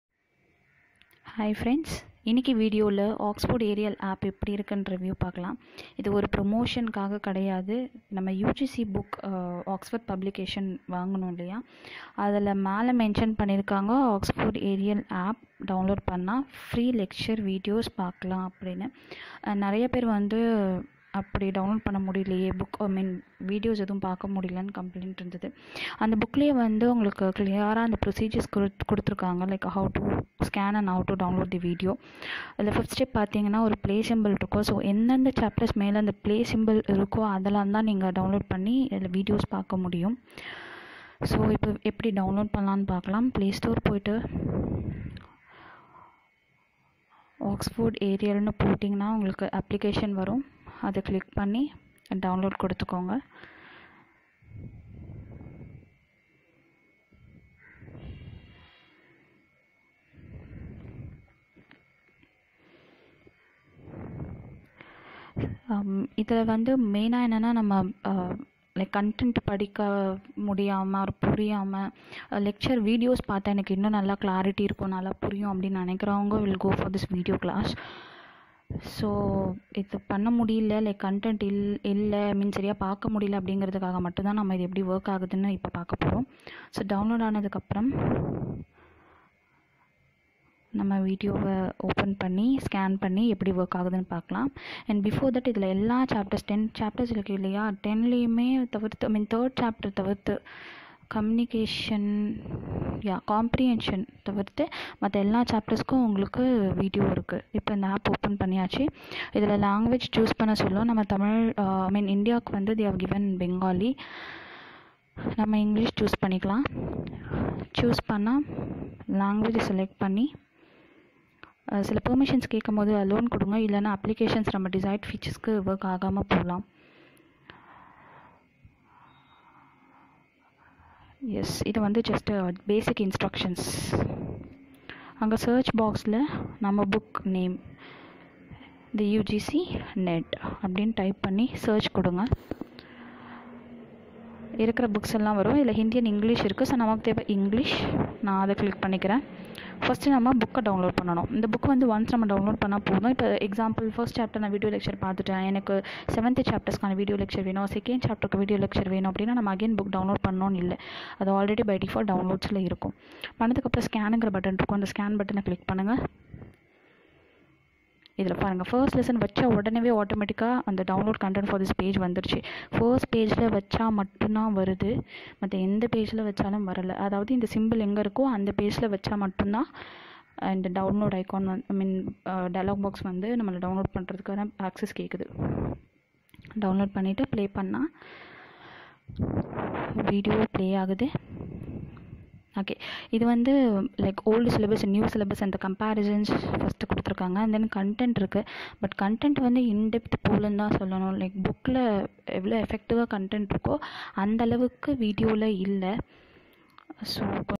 OXFORD AREAL APP FOR OXFORD PUBLICATION'S UGC NET 2019
This video gives you an idea to access oxford areal app to view free lecture video associated with each chapter in UGC NET OXFORD PUBLICATION TEXT BOOK